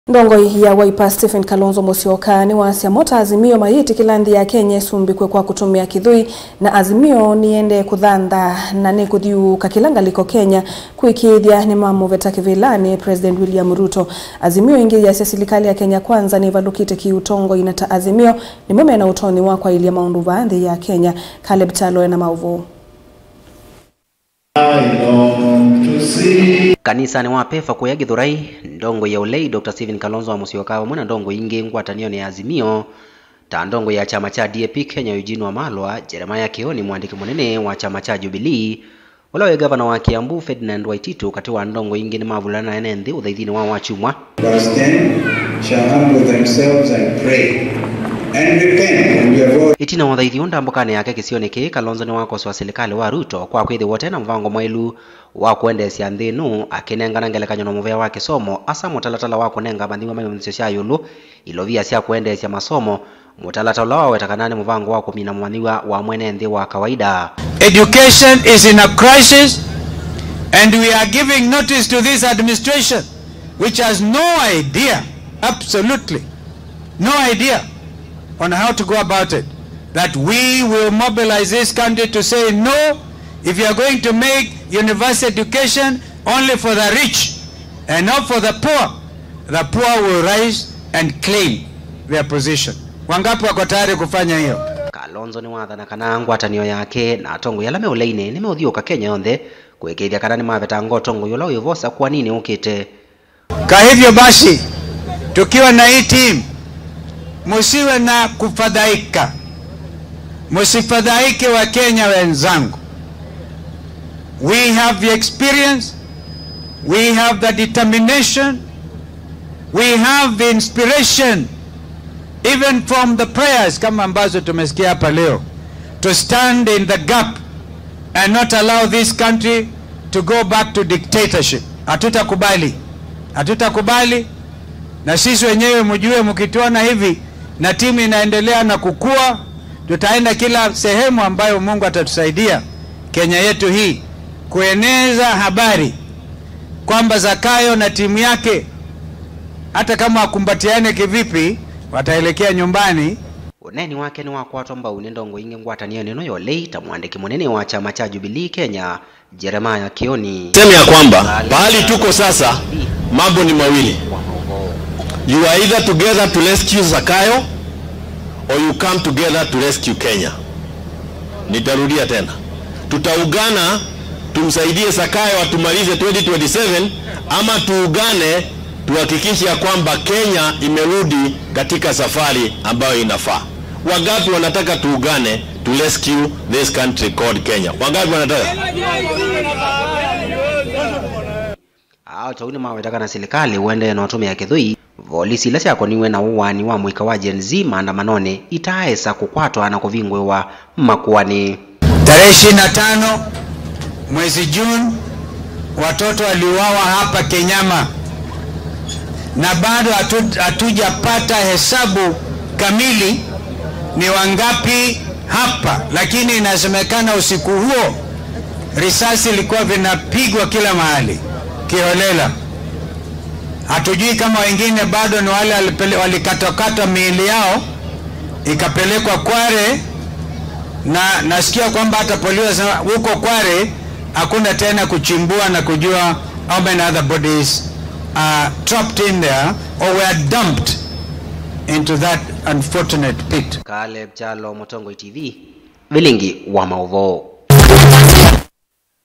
KALONZO MUSYOKA KUVUTHIISYA UTONGOI WA MUSUMBI WILLIAM RUTO
Ndongoi ya Wiper Stephen Kalonzo Musyoka niwasya mo ta Azimio mayiitikila nthi ya kenya isumbikwe kwa kutumia kithui na azimio niyiendee kuthanthaa na nikuthiuuka kila ngaliko kenya kuikiiithya nimamuveta kivilani musumbi William Ruto.